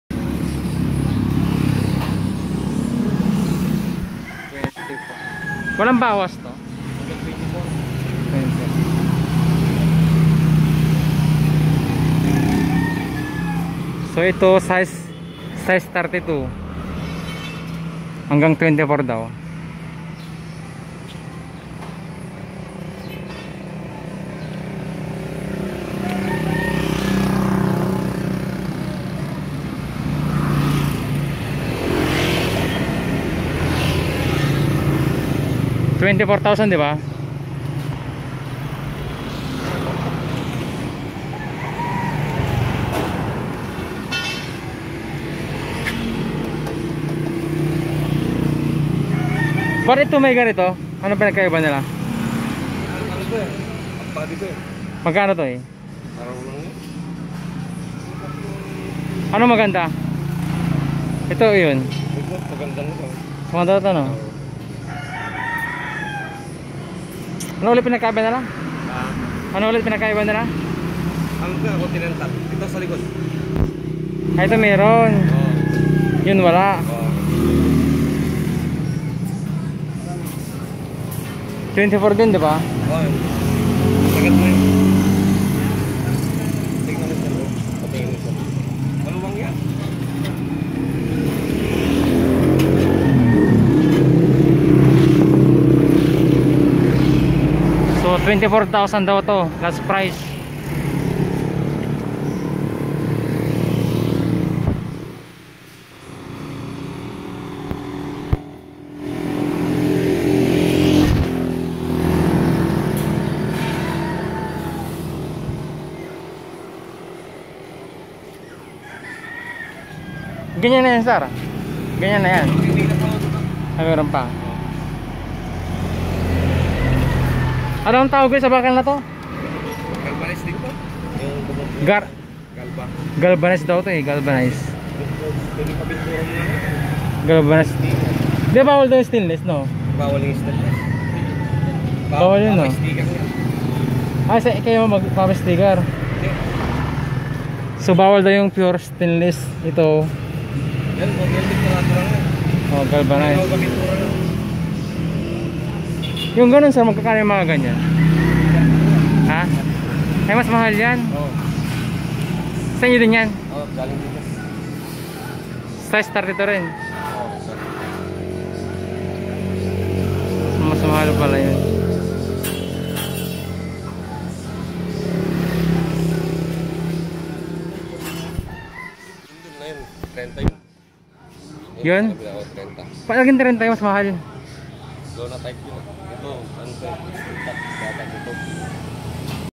BRAND NEW SIDECAR | FOR SALE | BUDGET Sidecar | 18k to 20k above | Pangasinan
24k only Tricycle Sidecar murang mura | Pangasinan